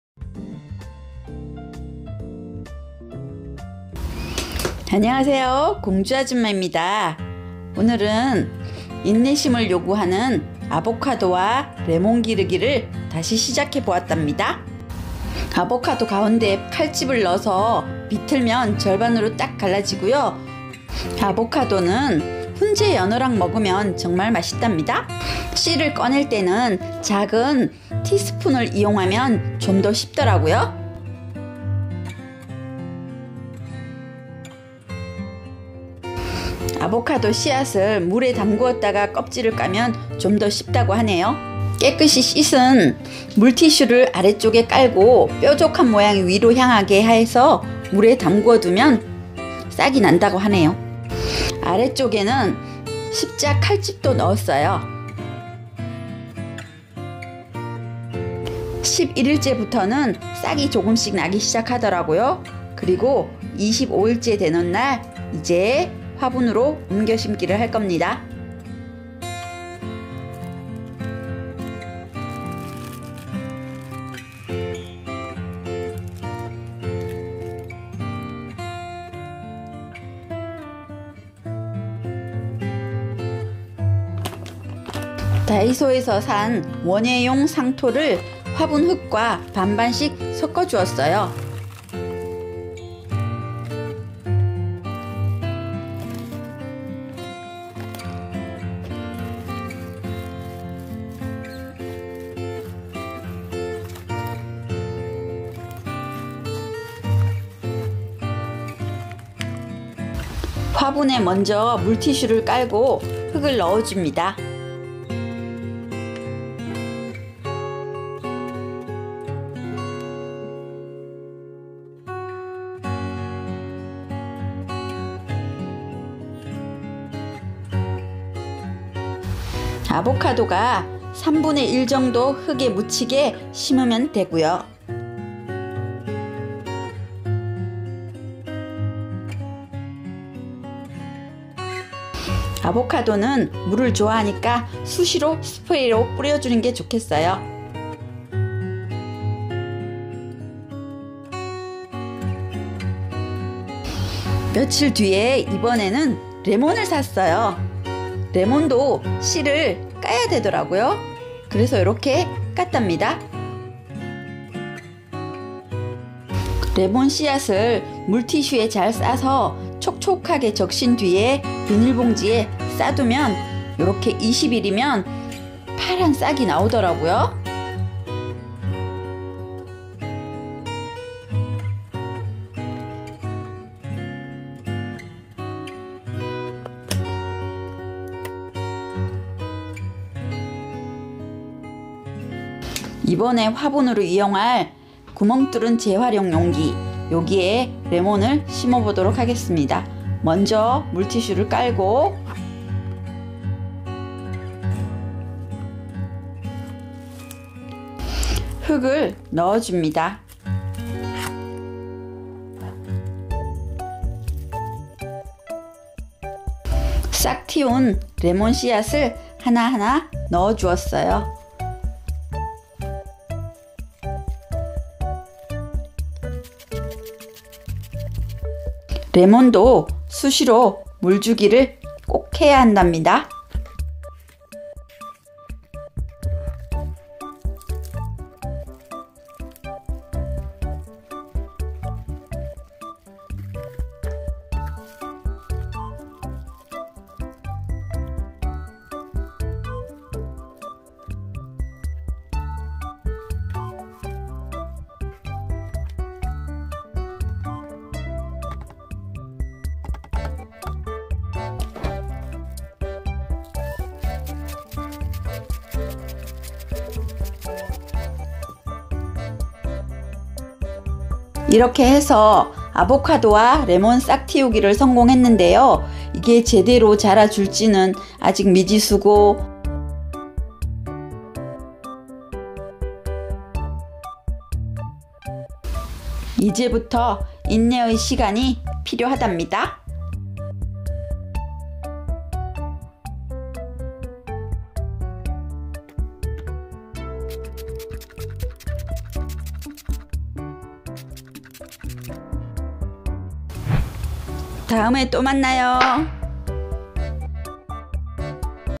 아보카도 열리는 날까지 기르기 기르는법 싹틔우기 레몬 기르기 발아 시간 인내의고통
열대과일 나무 아보카도와 레몬 기르기의 시작은 싹튀우기 발아하는 시간은 기다림의 연속이니 인내의 시간이지요. 아보카도 싹튀우기 하고 레몬도 발아시켜 함께 화분으로 옮겨 심었어요. 아보카도 발아시간은 25일  레몬도 쉽다는데 저는 20일이 걸렸네요.
아보카도와 레몬 잘 길러 볼게요!!